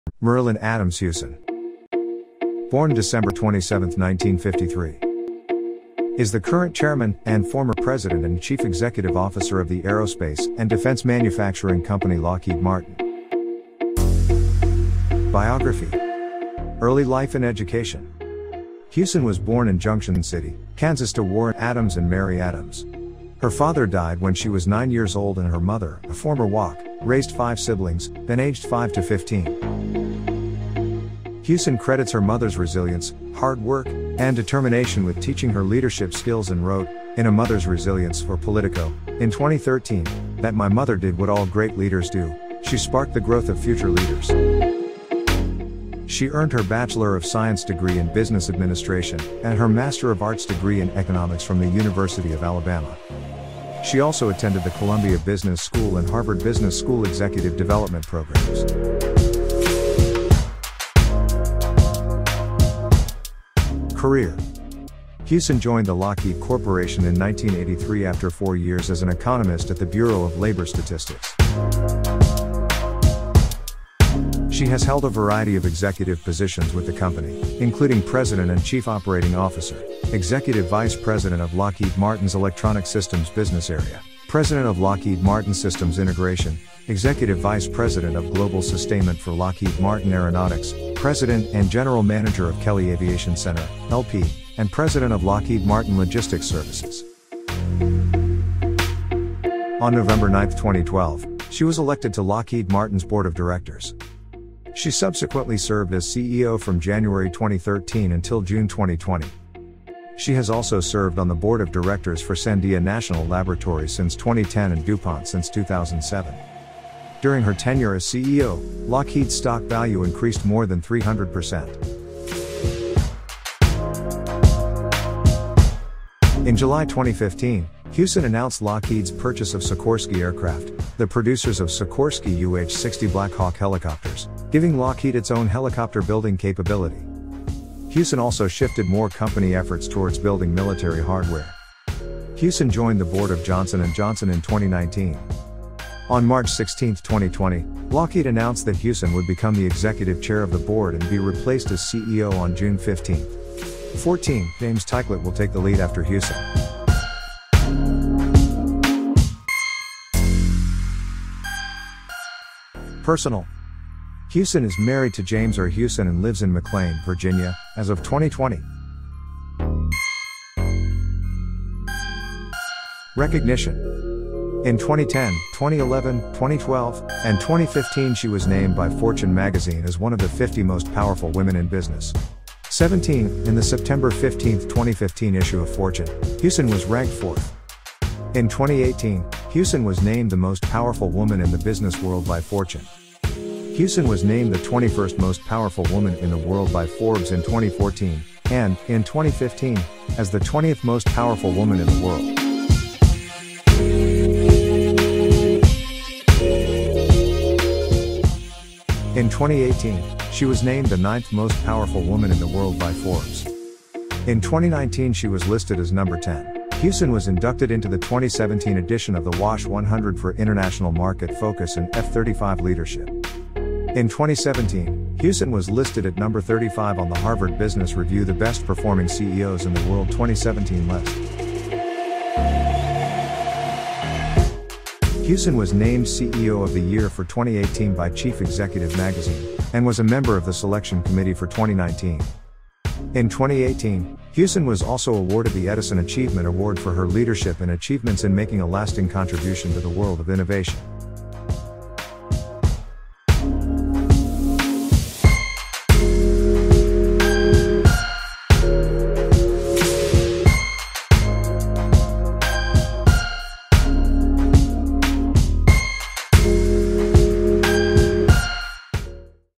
Marillyn Hewson - Powerful Woman
Marillyn Hewson - The Chief Executive Officer of Lockheed Martin
Marillyn Adams Hewson is the current chairman, and former president and chief executive officer of the aerospace and defense manufacturing company Lockheed Martin.